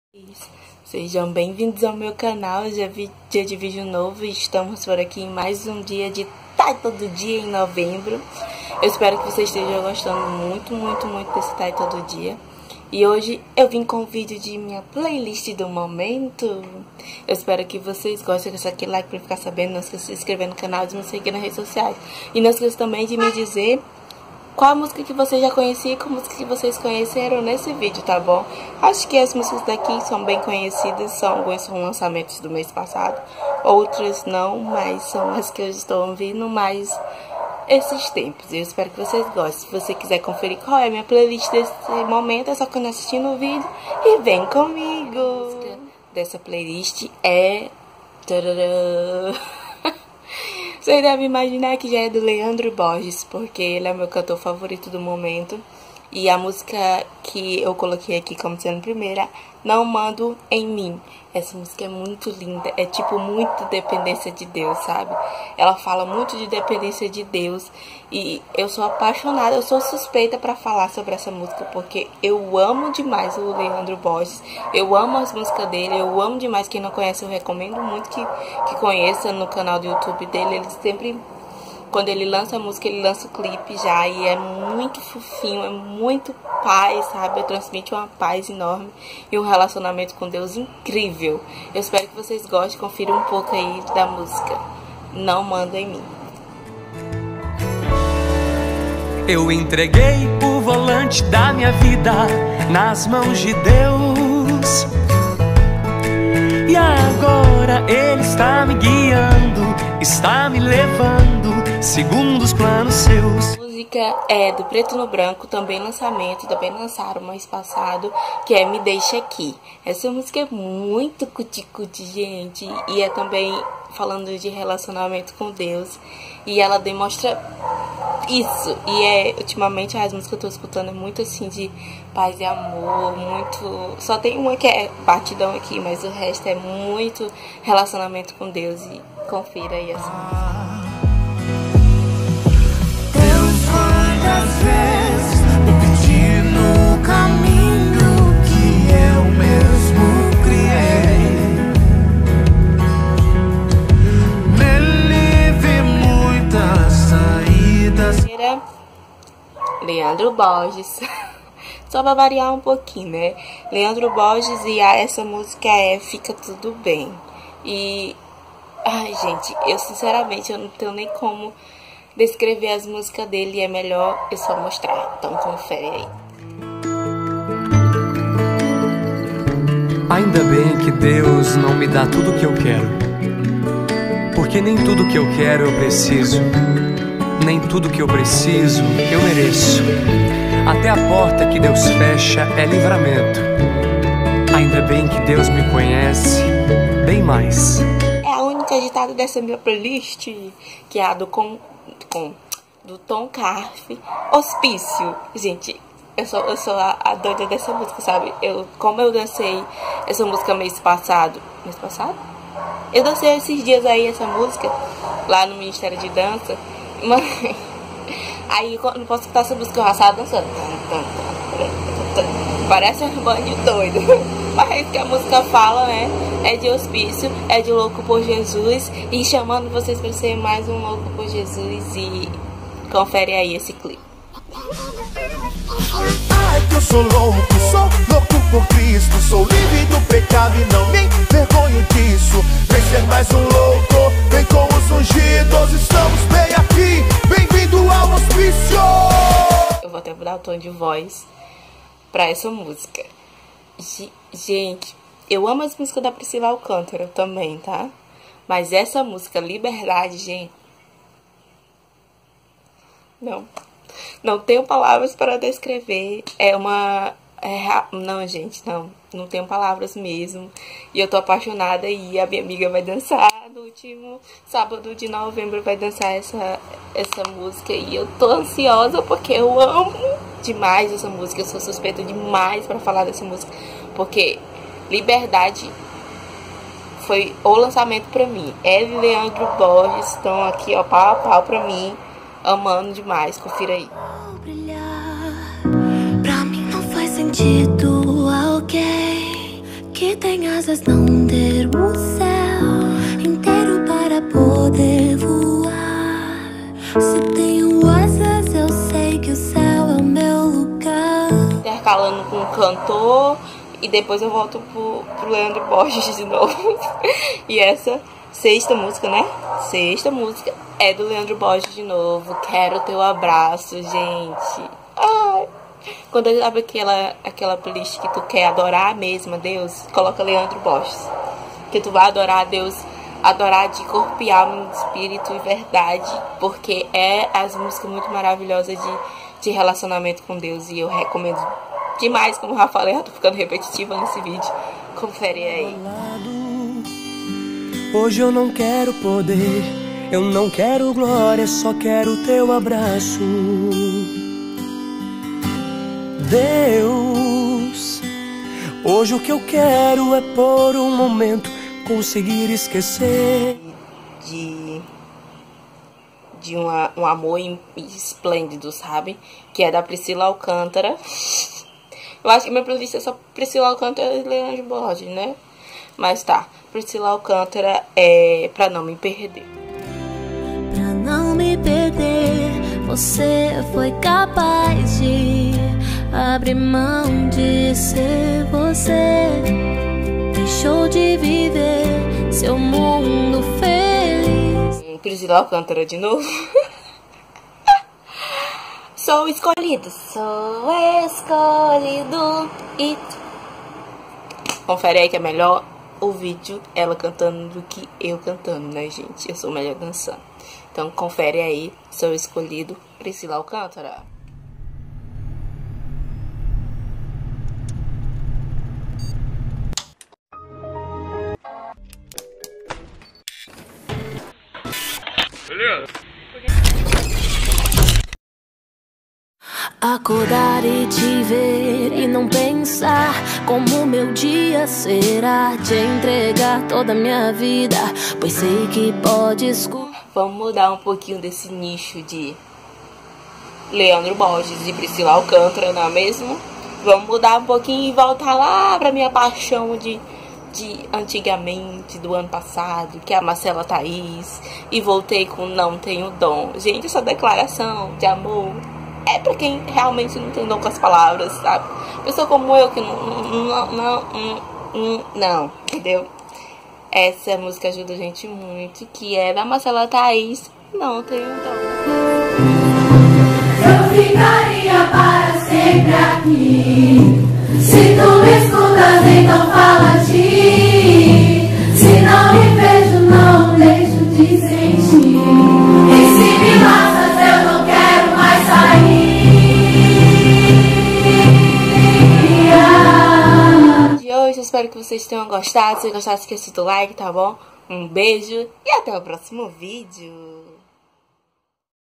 Playlist gospel do momento #06 por Thay Neri
Helloooo princeeesssss
Hoje eu trouxe as músicas que estou mais escutando ultimamente .

Fã page: cachos_cachinhosmeu